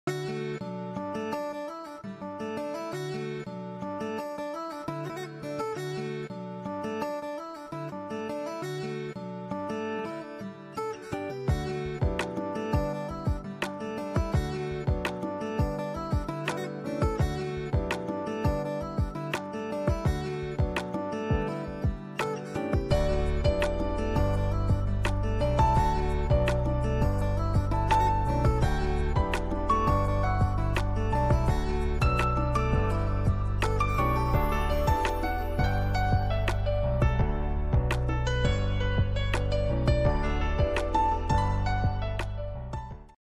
Vegan Pot Pie Soup
Curl up to a delicious cozy bowl of Vegan Pot Pie Soup! A thick creamy chowder is filled with veggies, vegan chicken, and fresh herbs. It’s just like the classic but in soup form! This comforting bowl of love is perfect for busy weeknights and one that everyone will devour.

Musician: Ikson Music